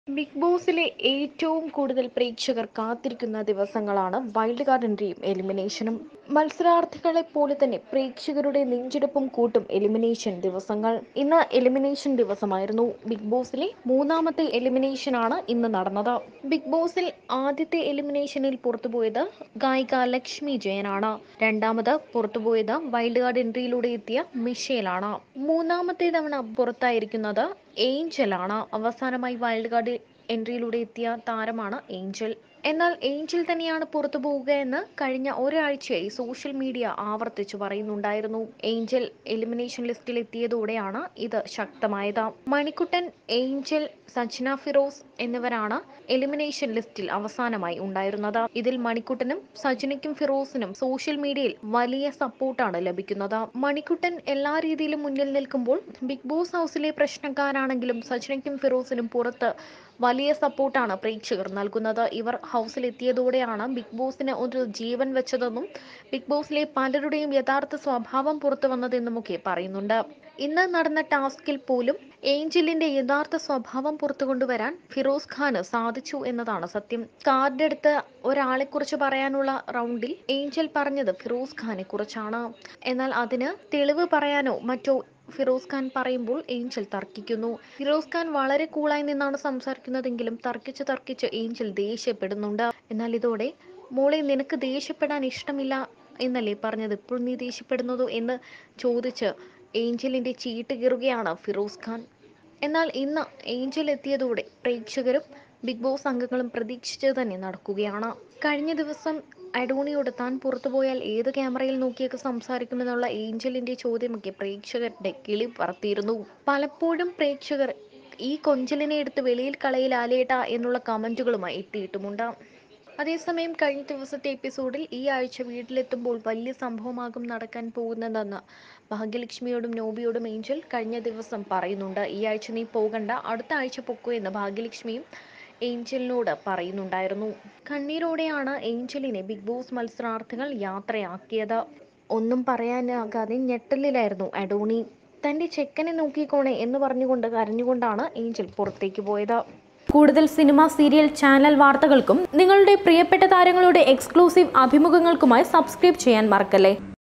എയ്ഞ്ചല്‍ തോമസ് ബിഗ്‌ബോസിൽ നിന്നും എലിമിനേറ്റ് ആയി പുറത്തേക് l Angel Thomas l Bigg Boss Season 3
എയ്ഞ്ചല്‍ ബിഗ്ബോസില്‍ നിന്ന് പുറത്ത്...
പ്രേക്ഷകര്‍ പ്രവചിച്ചത് നടന്നു...!